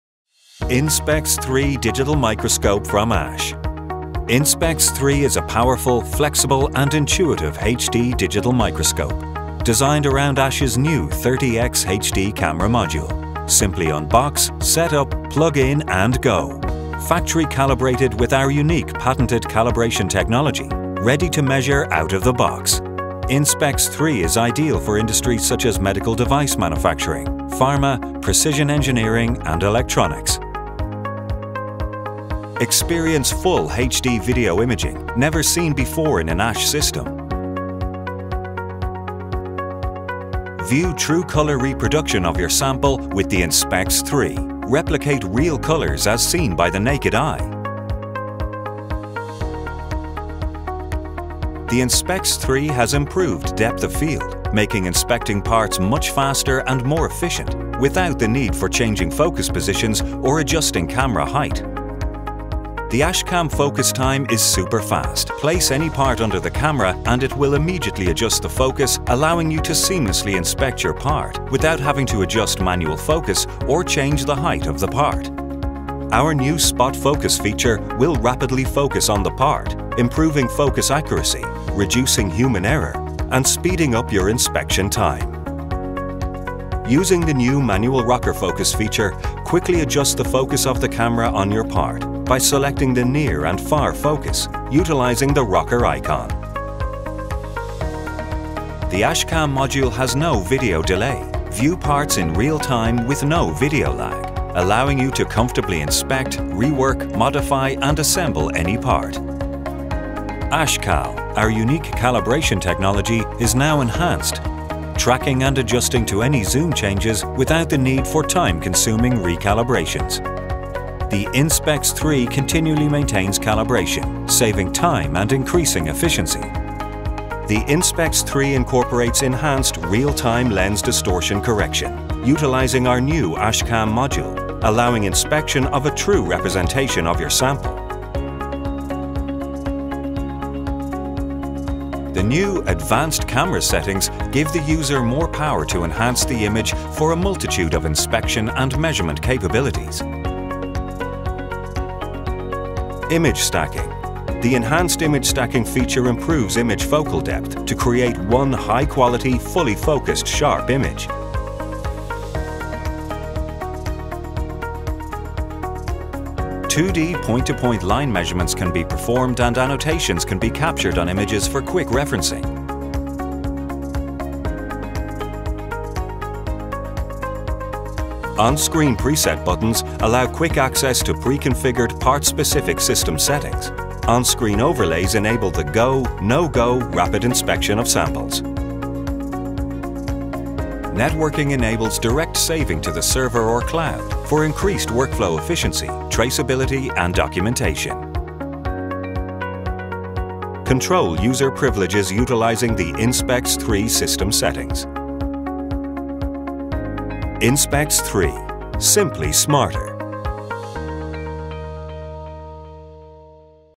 Inspex 3 Overview
The Inspex 3 is a full HD (1080p) digital microscope inspection system that operates without a PC. The Inspex 3 platform has a powerful image processing and control engine that delivers superb full live video image quality at 60 frames per second. This enables the system to be utilized as a solution in a broad range of quality control, testing,  assembly, inspection and documentation tasks.  It is ideal for production and quality control environments. The Inspex 3 provides efficiency, control and traceability of inspection.

Several new key features deliver faster, more reliable results:
● 3 New Focus Modes:
  ◽ Superfast Auto-Focus instantly focuses on the part being inspected.
  ◽ Rocker Focus toggles between two focus points.
  ◽ SpotFocus™ instantly focuses with the click of the mouse.
● RTLDC™ & AshCal™: The patent-pending Real Time Lens Distortion Correction and AshCal factory precalibration ensure that every image is ready for the most exacting measurements.
● AshTruColour™ ensures that the images are crisp, brilliant, and reflect the true color of the sample.
● Inspex 3 has No Video Latency allowing operators to inspect, assemble, rework, or modify any part without a delay in the video display.
● Fully-integrated and complete solution: Inspex 3 is complete and ready to go right out of the box. Just add an HD monitor.
● New KP3 key pad allows rapid camera and illumination control.

Inspex 3. Simply Smarter.